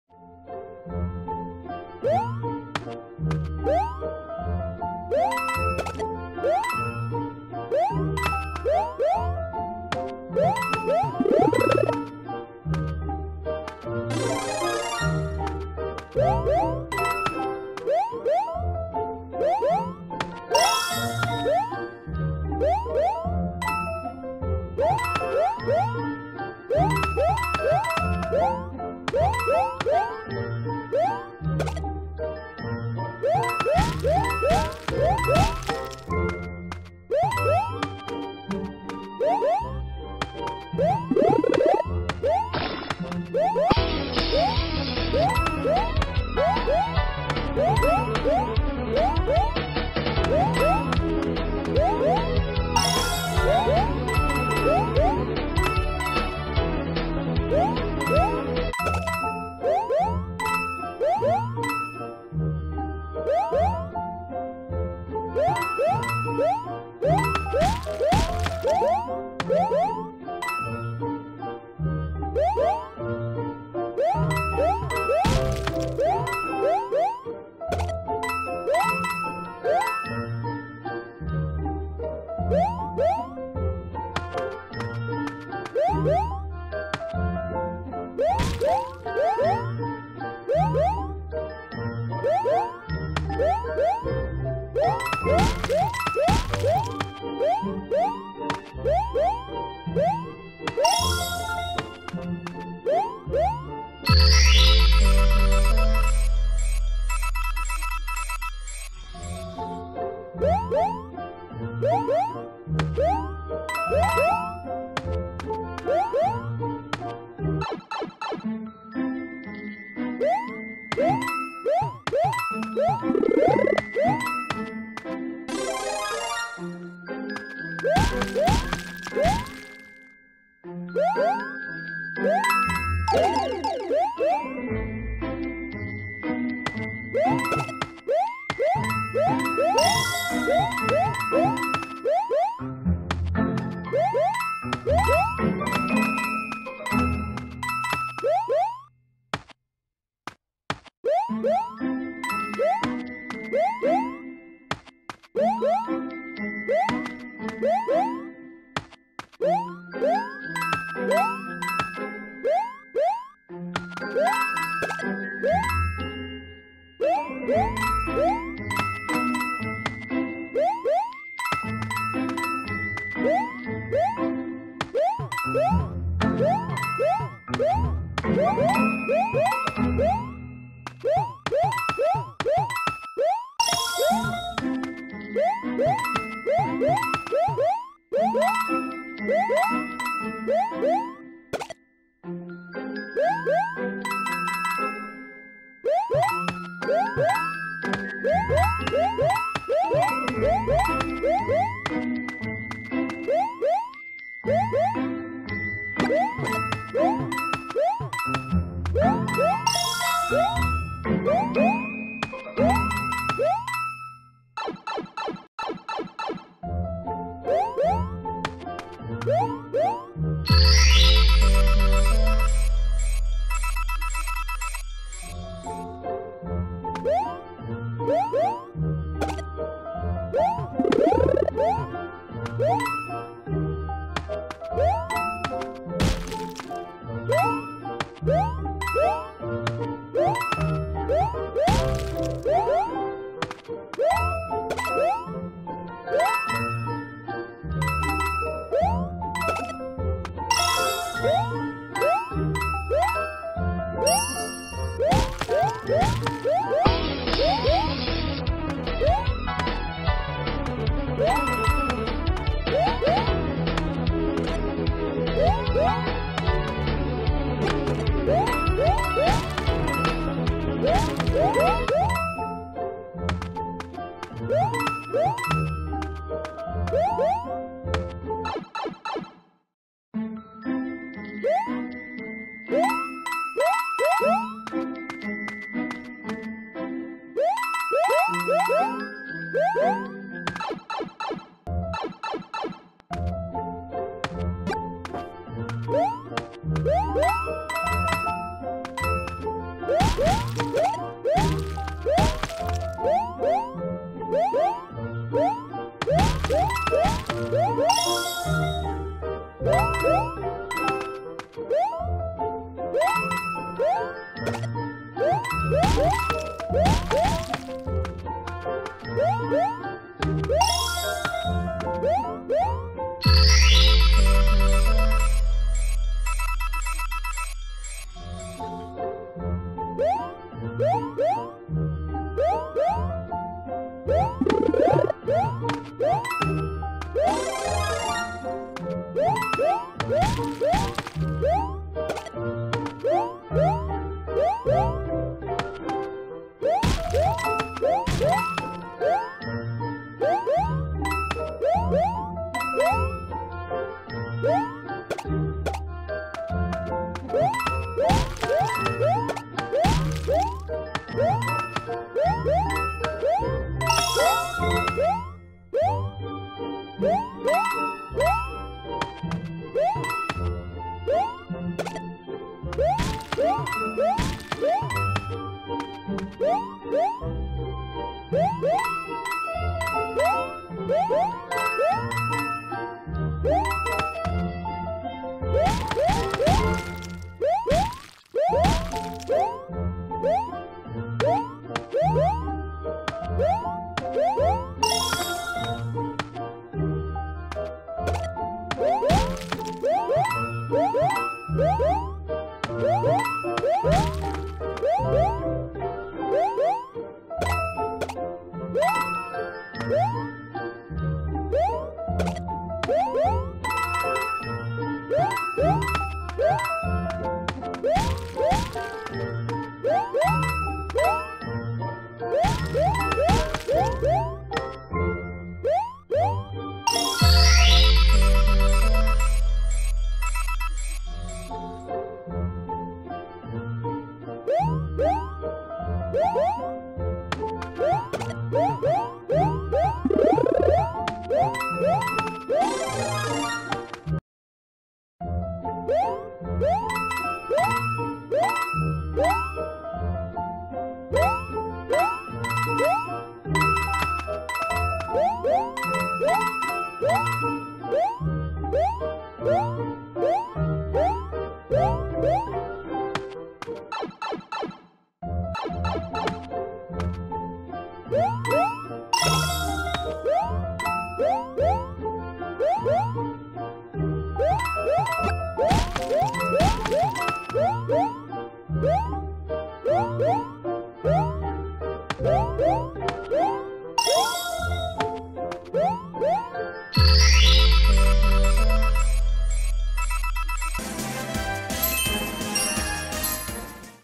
Super Matteo - Aventuras Peligrosas de Super Matteo Cap. 1 - Juegos para Niños
Chicos vamos a divertirnos con las aventuras de Super Matteo donde tendremos carreras y aventuras impresionantes.